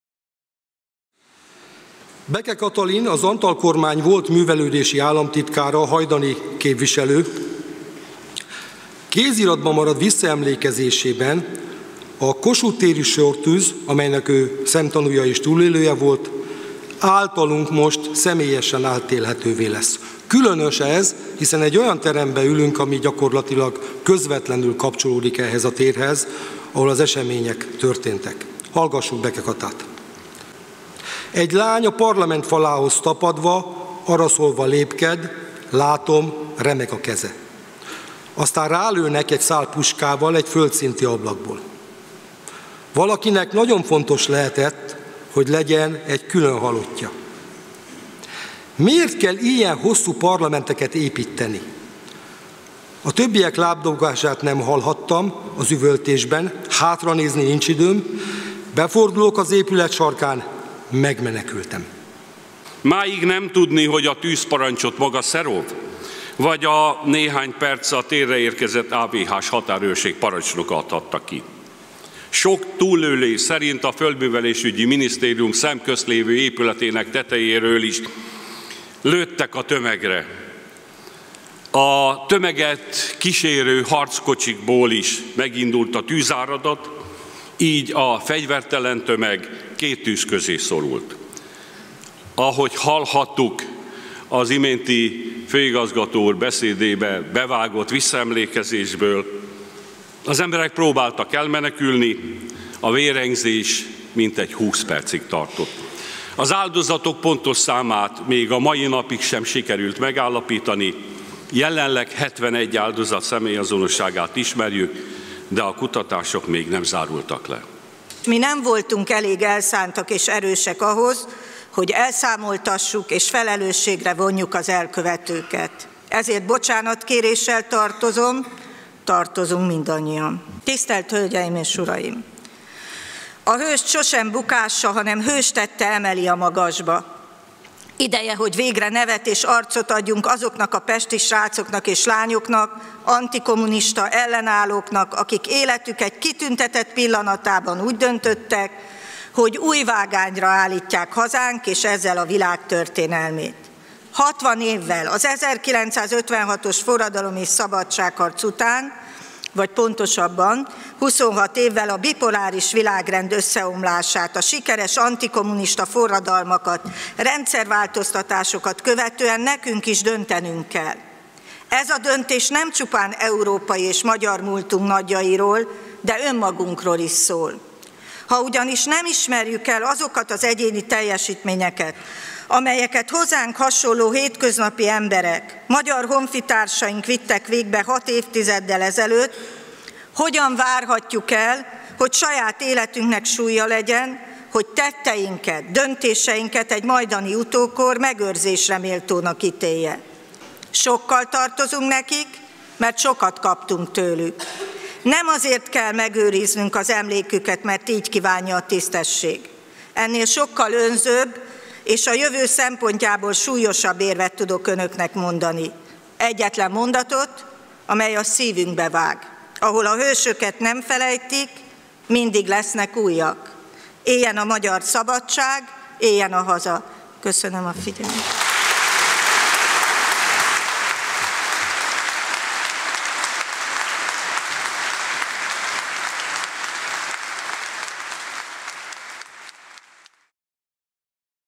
Schmidt Mária: Az október 25-i sortűz felelőseit a mai napig nem vontuk felelősségre
A Magyar Országgyűlés felsőházi termében ünnepi ülést tartottak az 1956-os forradalom és szabadságharc tiszteletére, majd szakmai konferenciával emlékeztek az október 25-i Kossuth téri sortűz áldozataira. Az ünnepi ülésen felszólalt Kövér László, az Országgyűlés elnöke, Marek Kuchcinski, a lengyel szejm elnöke valamint Stanislaw Tillich, a német Bundesrat elnöke. A szakmai konferencián előadást tartott Schmidt Mária, az '56-os Emlékév kormánybiztosa, Latorcai János, az Országgyűlés alelnöke és Földváryné Kiss Réka, a Nemzeti Emlékezet Bizottságának elnöke.